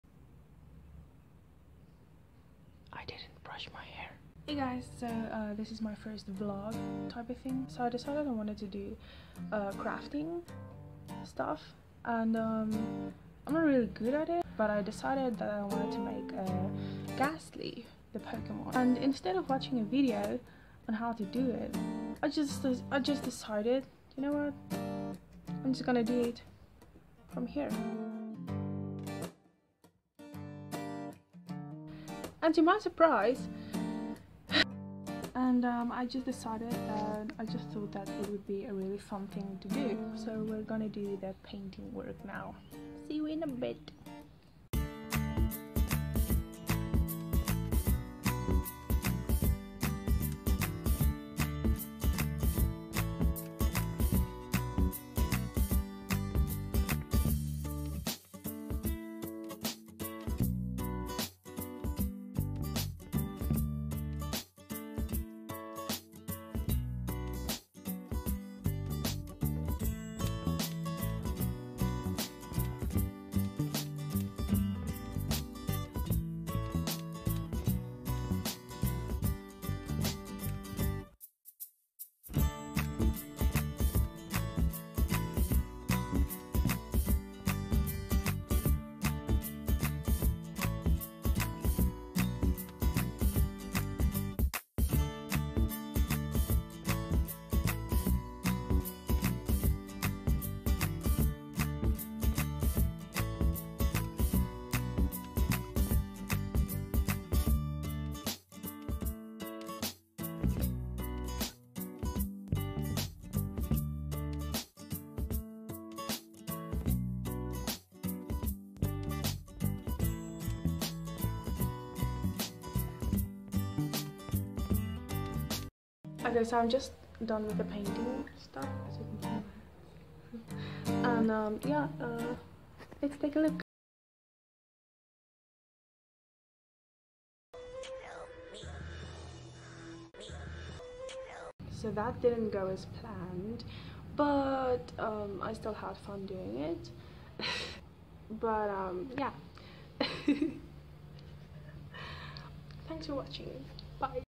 CRAFTING A POKEMON - Lets make a Gastly!
Welcome to my First ever Vlog!

This was literally just for fun so i decided why not record it.